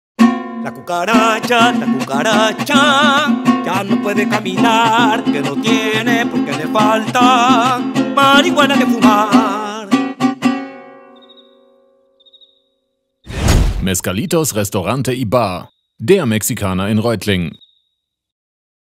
Mezcalitos Restaurante y Cocktail Bar Reutlingen - Der Kinospot
Unser aktueller Kinospot! Mit Mariachi Lizardo Bonilla, der in Wirklichkeit natürlich brillant singen und Gitarre spielen kann ... Regie: Dominik "Dodokay" Kuhn.
Mehr Info über's Mezcalitos Mexican Restaurante y Cocktail Bar in Reutlingen